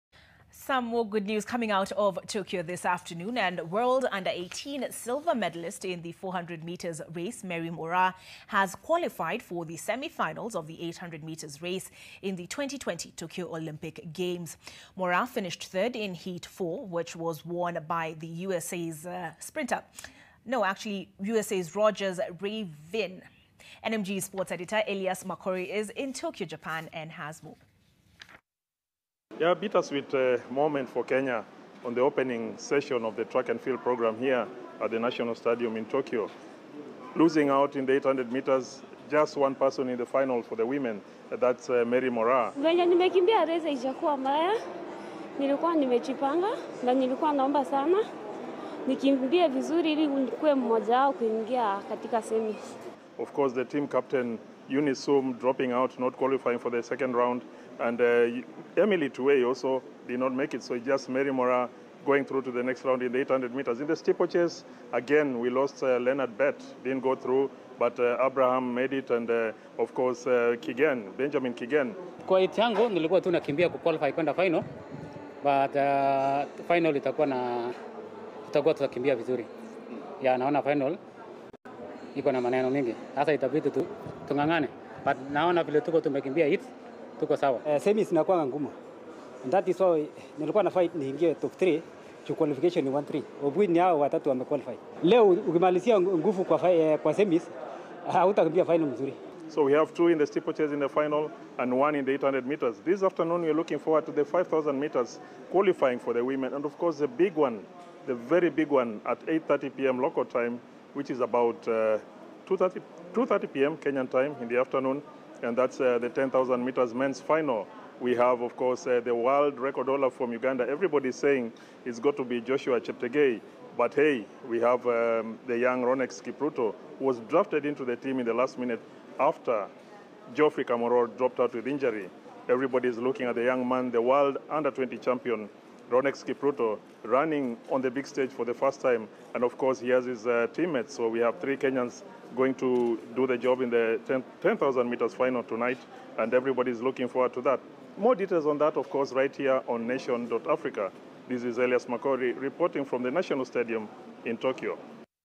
Mary Moraa qualifies for semifinals of 800 metres in the 2020 Tokyo Olympic Games
World Under 18 silver medalist in the 400 metres race Mary Moraa has qualified for the Semifinals of the  800 metres race in the 2020 Tokyo Olympic Games.